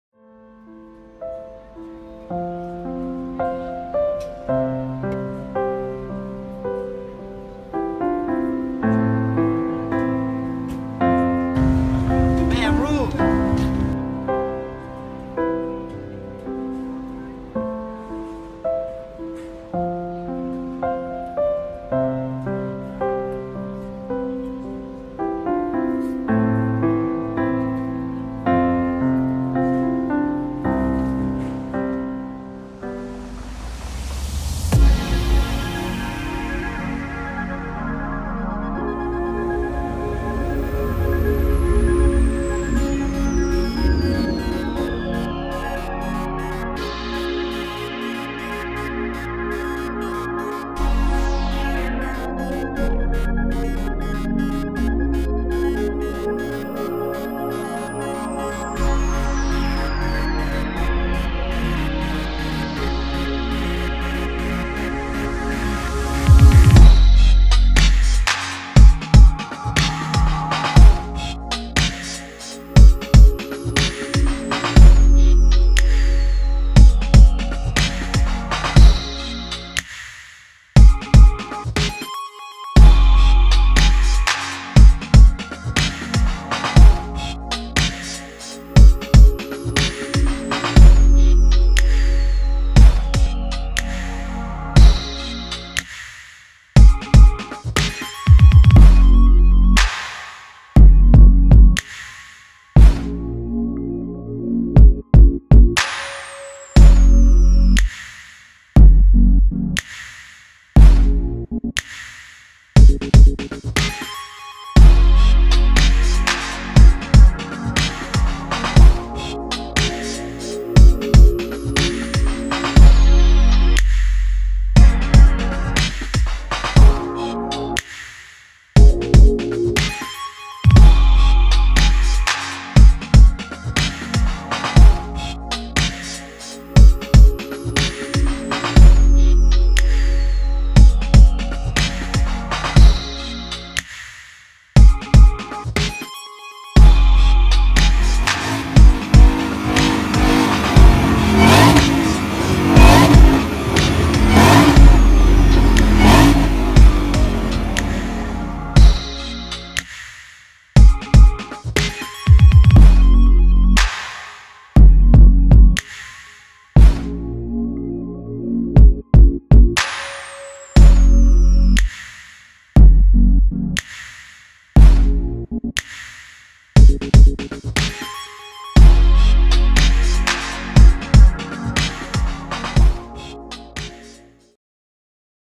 Mustang Mafia - August 2016 Meet
Mustang Mafia's first meet was a great success.  We raised over 1000.00 in less than two hours to benefit the flood victims in Baton Rouge.  Every event from that point on will go to benefit a charitable organization so if you live in the southern Louisiana area keep your eyes open for one of hour events!